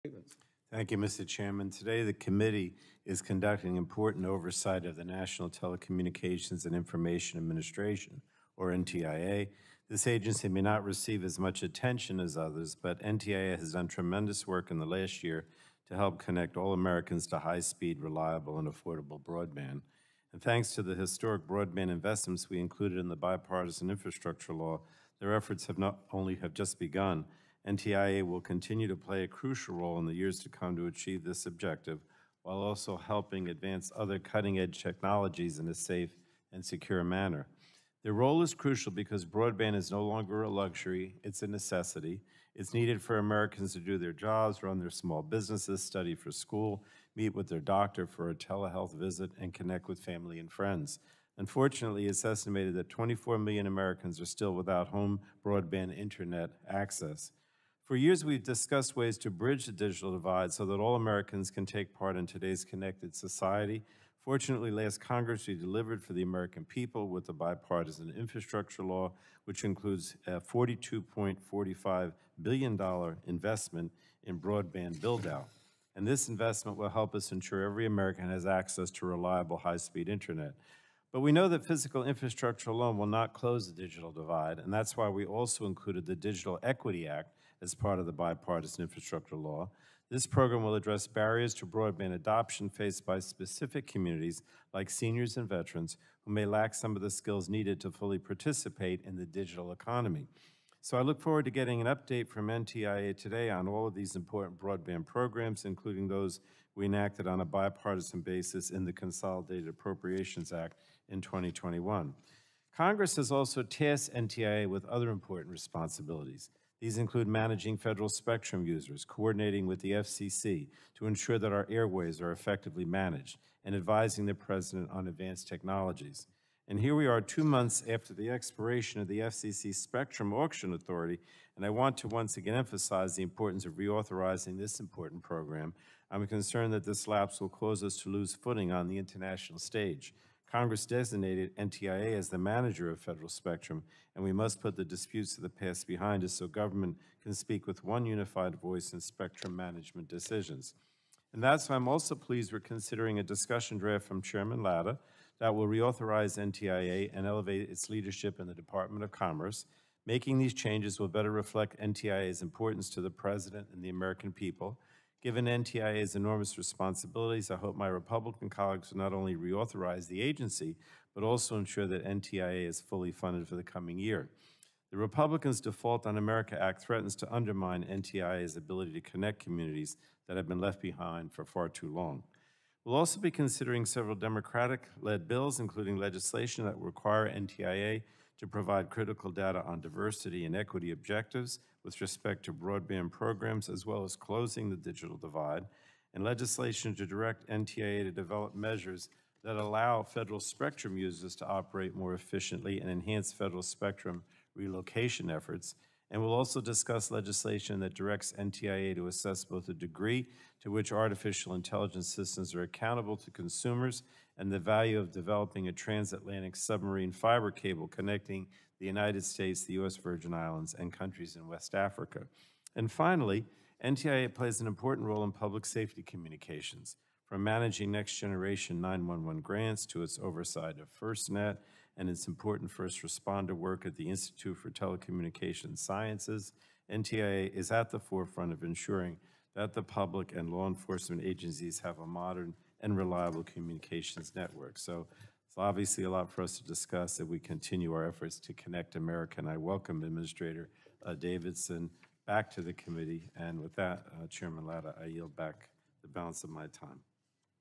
Pallone Remarks at Hearing on Oversight and Reauthorization of the NTIA
Energy and Commerce Committee Ranking Member Frank Pallone, Jr. (D-NJ) delivers remarks at a Communications and Technology Subcommittee legislative hearing titled, “Oversight and Reauthorization of the National Telecommunications and Information Administration:”